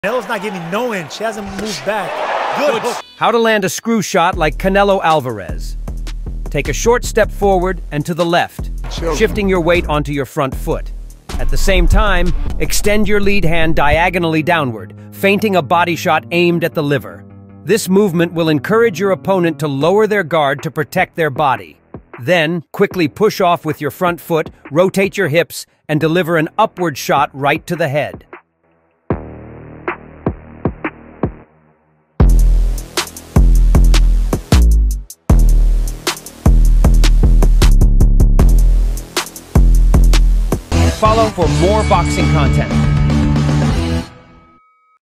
Upward hook or screw shot by Canelo Alvarez.
Upward lead hook or a screw shot tutorial.

How to perform a screw shot like Canelo Alvarez:
Take a short step forward and to the left, shifting your weight onto your front foot.
At the same time, extend your lead hand diagonally downward, feinting a body shot aimed at the liver.
This movement will encourage your opponent to lower their guard to protect their body.
Then, quickly push off with your front foot, rotate your hips, and deliver an upward shot to the head.

FOLLOW FOR MORE BOXING CONTENT